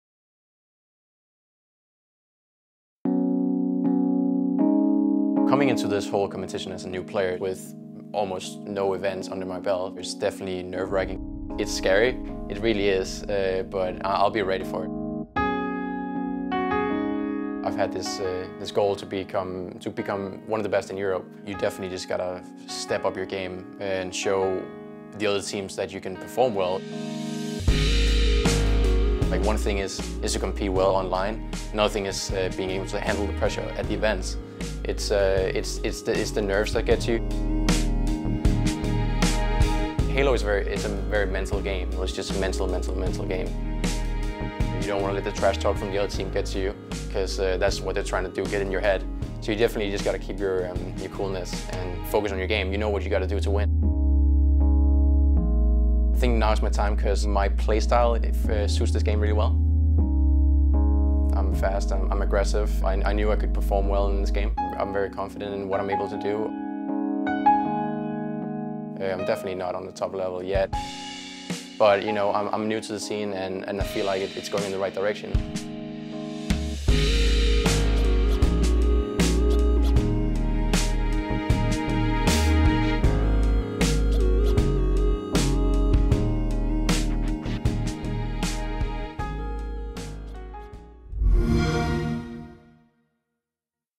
Doodle: Til Valhal - Halo World Championship 2016
Despite being a relative newcomer to the professional Halo scene, Daniel “Doodle” Peterson is eager to prove that his talented European squad at FABe has what it takes to shock the world and take the Halo World Championship crown.

For more Halo World Championship coverage including the latest schedule, results and stats GO